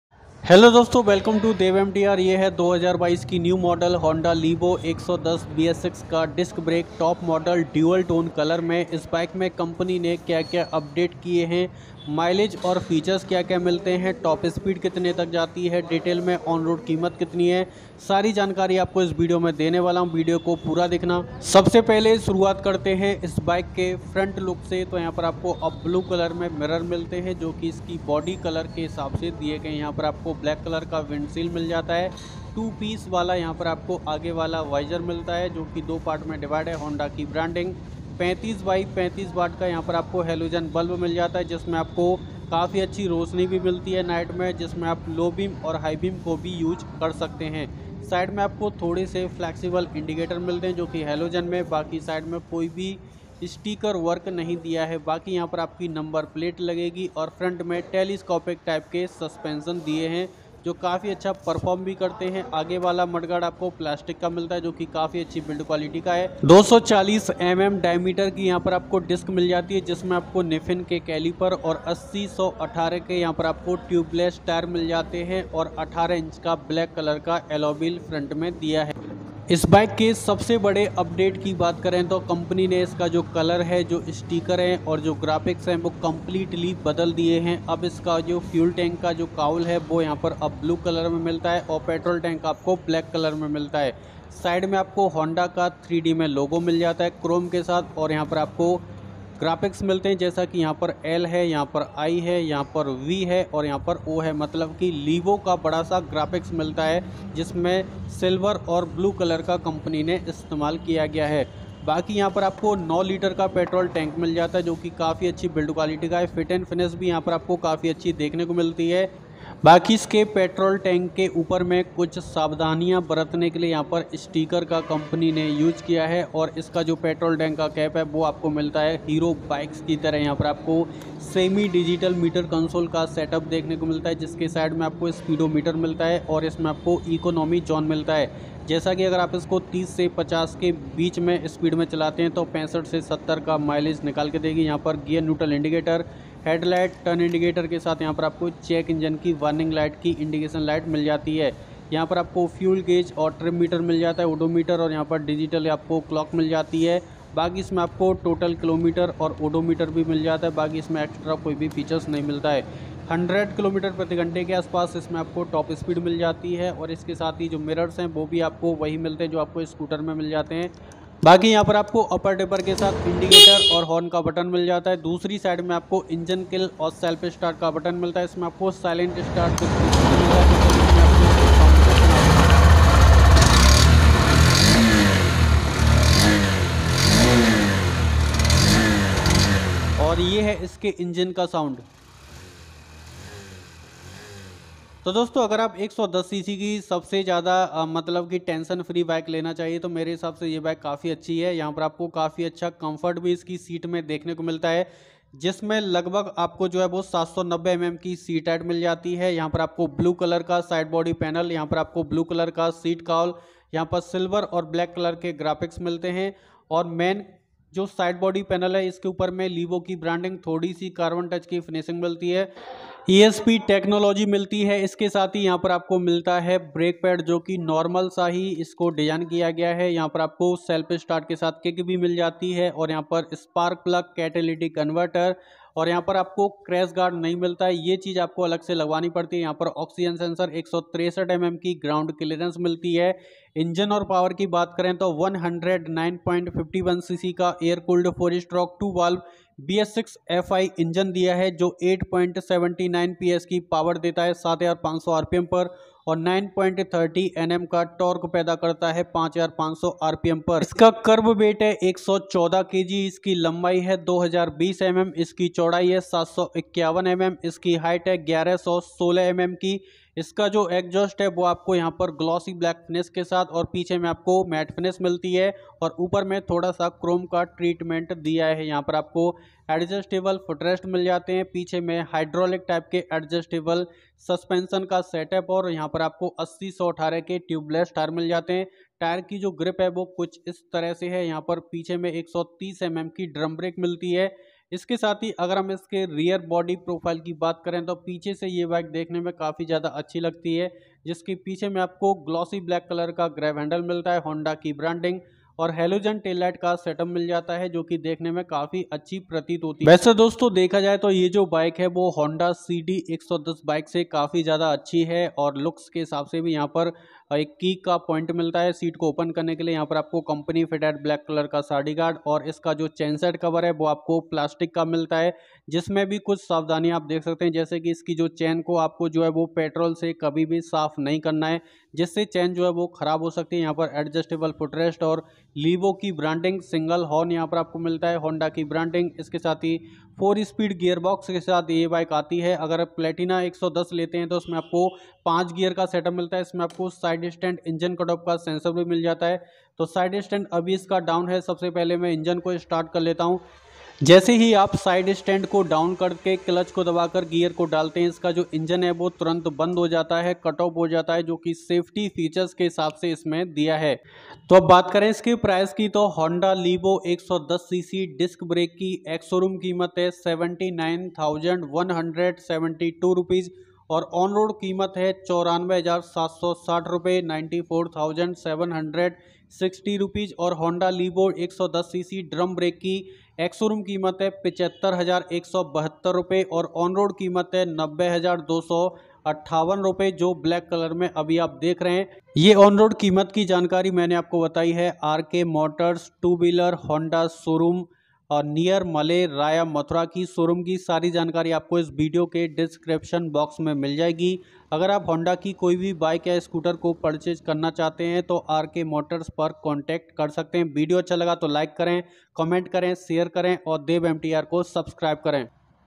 Ye Wali Hai All New 2022 Honda Livo 110 Details Review | on road price New Update Features
Showroom Details -
  RK MOTORS HONDA Showroom
 Add - Vill Malleh Near Tomar Guest House , Mathura Road Raya Mathura ( Uttar Pradesh )

Dosto Is Video Me Mene Honda Livo Disc Brake Ka Review Kiya Hai Jisme Mene Iske On Road price Features Mileage  Ke Bare Me Bataya Hai I Hope Ye Video Apko Acha Lagega So Video Ko Like Aur Share Karen.

Topic Cover -

1. honda livo 110 on road price

2. honda livo top speed

3. Livo 110 disc brake

4. Livo mileage

5. honda livo price